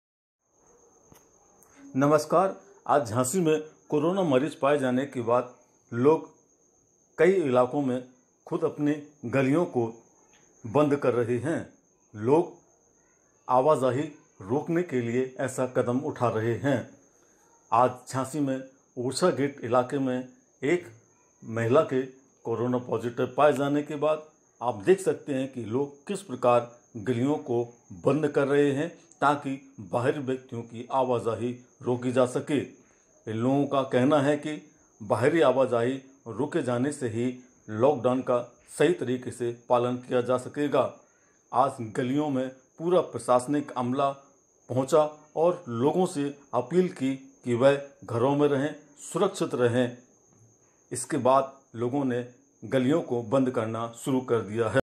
झाँसी की गलियों में लगने लगा पहरा, बाहरी लोगों का प्रवेश किया ऐसे बंद
झाँसी मैं एक महिला के कोरोनावायरस पाए जाने के बाद झांसी के अन्य इलाकों में लोग सुरक्षा के कदम उठाने लगे हैं लोग अपने मोहल्ले की गलियों को बाहरी व्यक्तियों के प्रवेश के लिए बंद कर रहे हैं